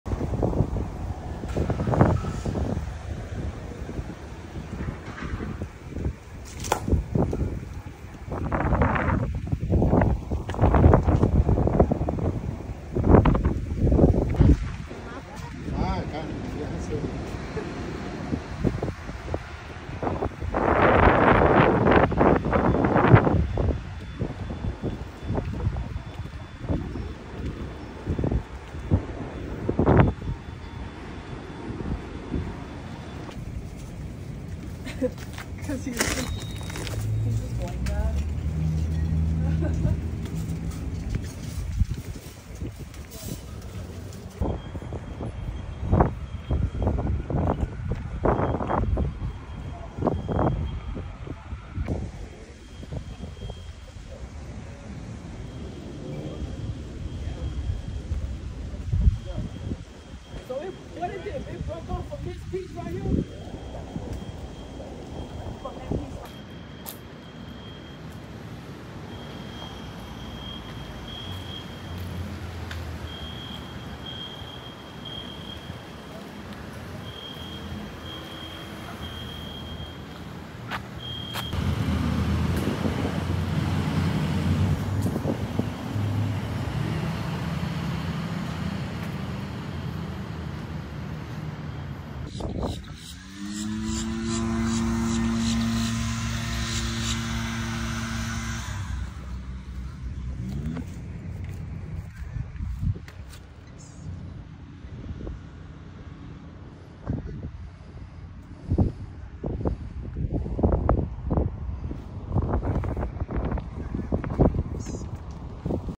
Hurricane Milton Leaves Widespread Damage in St Petersburg
Damage from Hurricane Milton could be seen on the streets of St Petersburg, Florida, on October 10, following the storm’s landfall the previous day.
Credit: Will Mitchell via Storyful